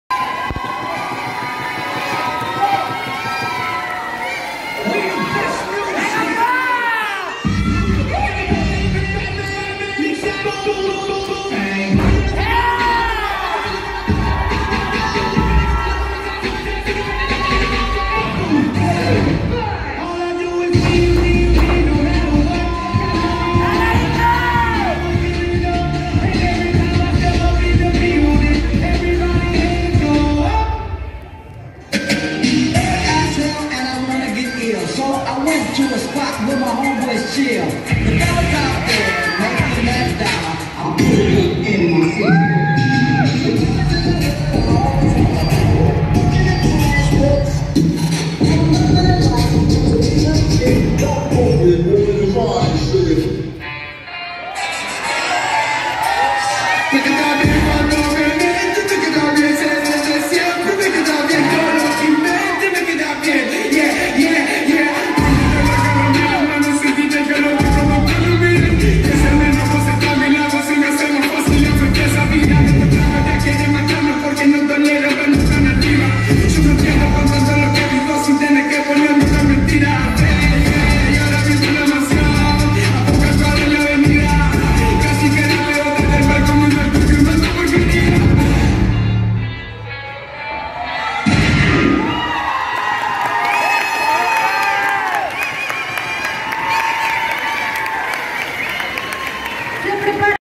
Emilio Gamboa Royalti
Presentación de Emilio Gamboa de Royalti Academy en la Categoría de Hiphop avanzado Solo en OJD MTY REGIONAL 2022

No tengo los derechos de la música del autor